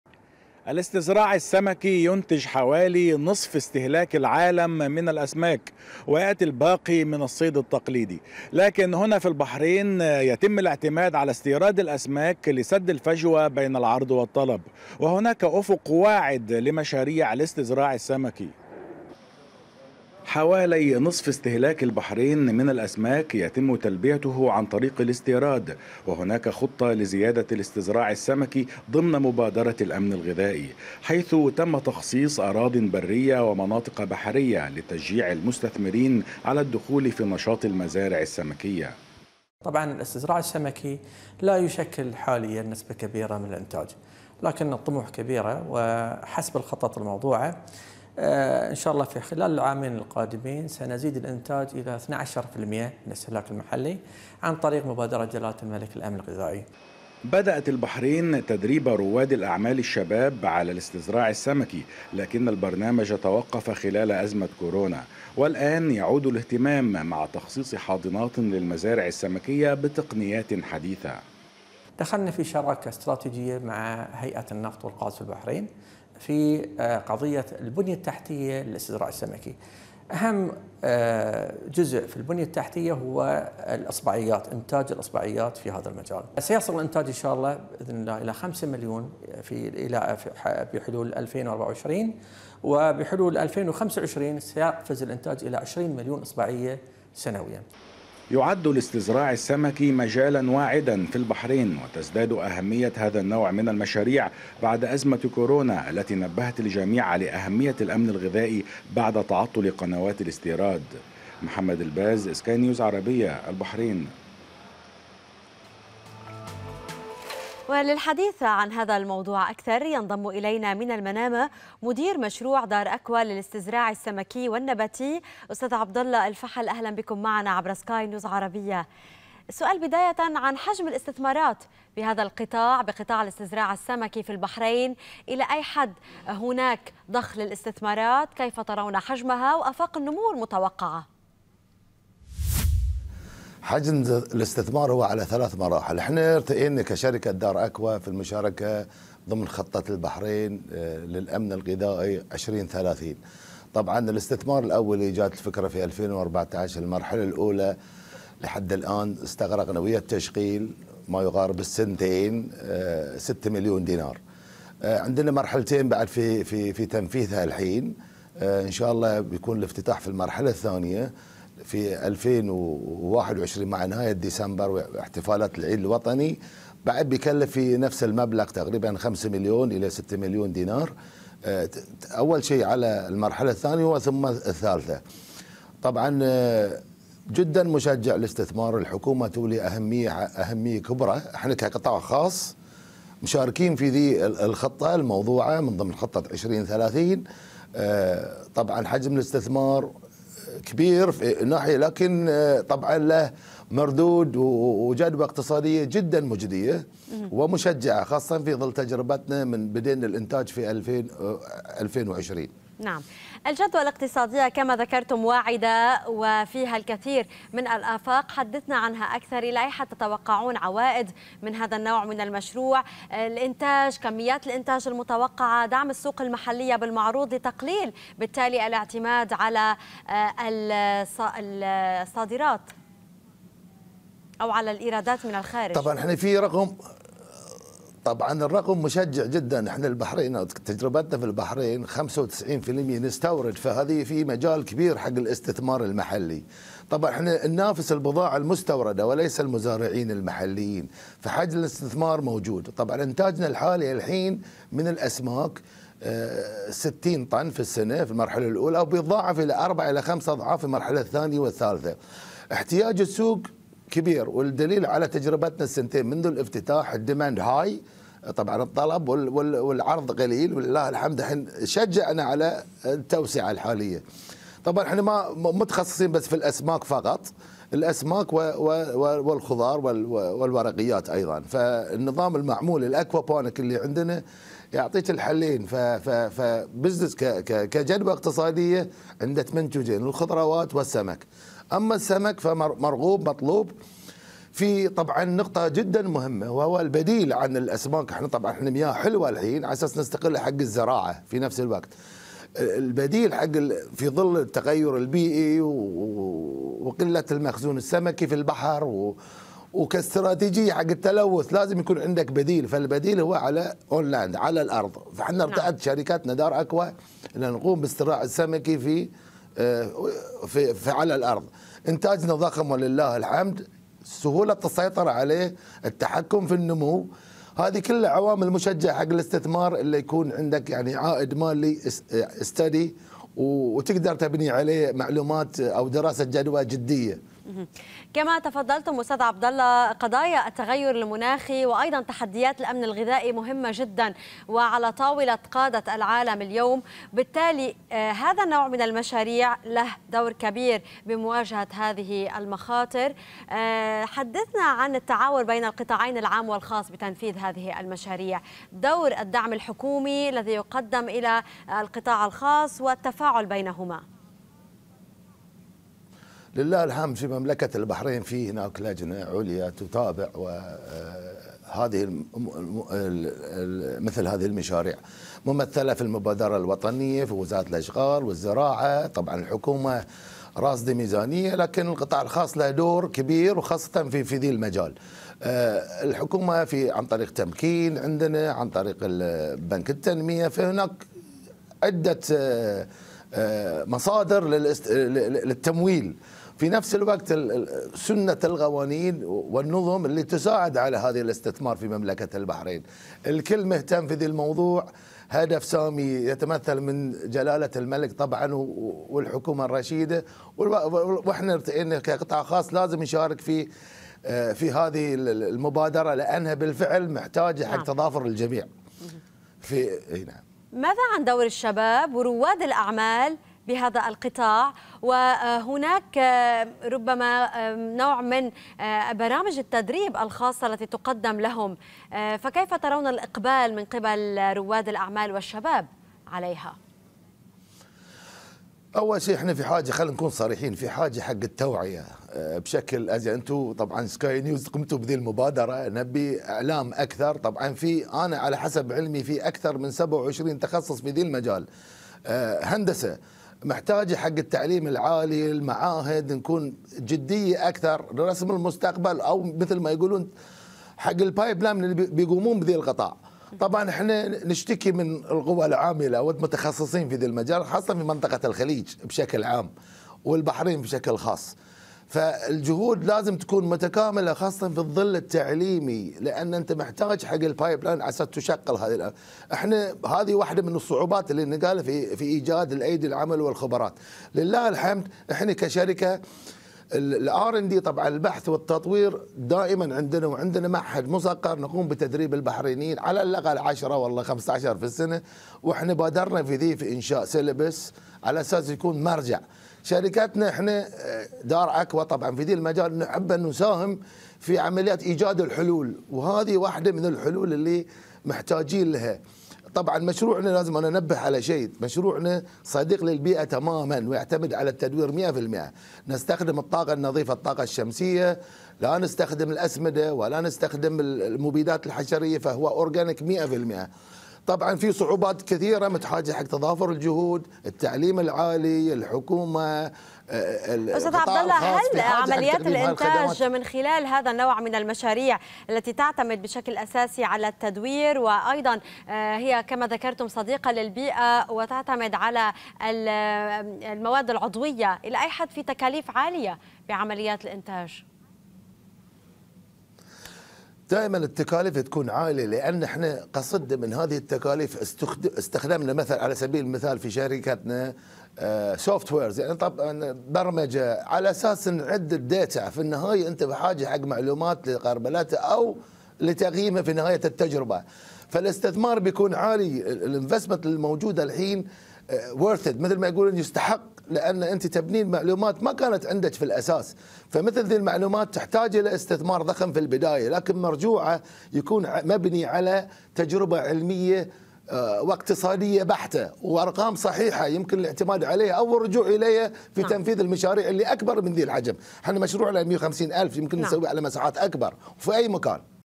مستقبل واعد لمشاريع الاستزراع السمكي بالبحرين
تستهدف البحرين زيادة الاستزراع السمكي خلال العامين القادمين ليصل إلى 12٪ من الاستهلاك ضمن خطة حكومية للأمن الغذائي و تأسيس شركات جديدة مع إعطاء تسهيلات ودعم لرواد الأعمال